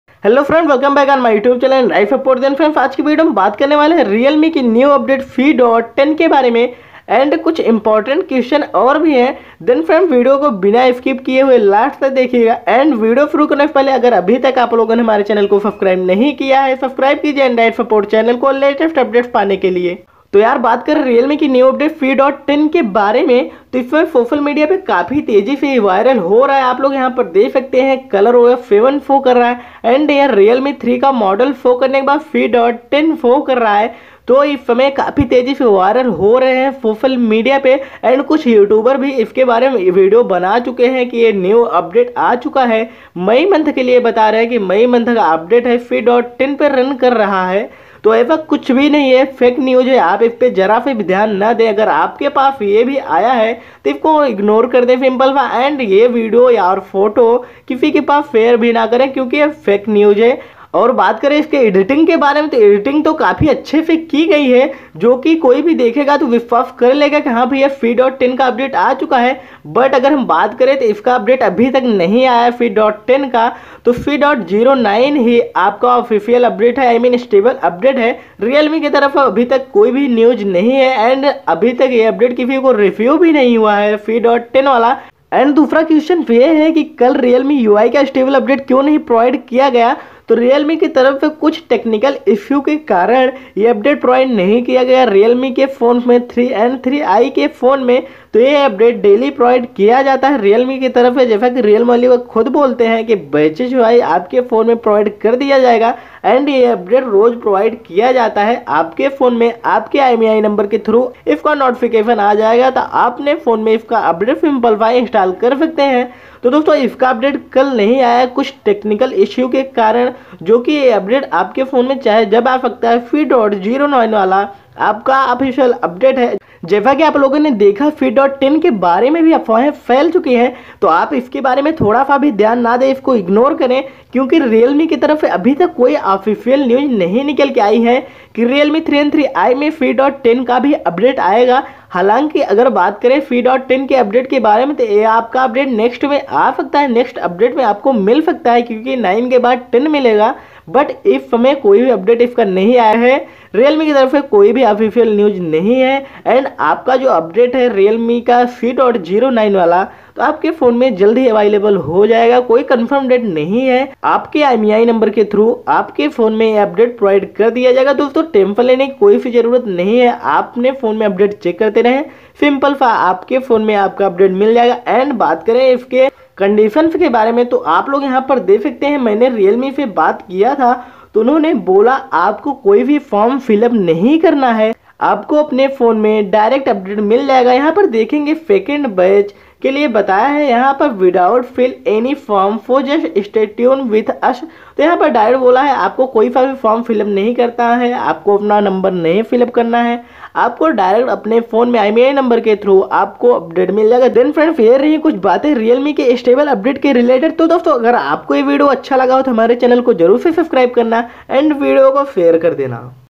Realme UI C.10 New update available in Realme 3||Realme UI update 3&3i device
Realme UI New update available for Realme 3 and Realme UI stable update rolling out soon... Thanks

Join my second WhatsApp group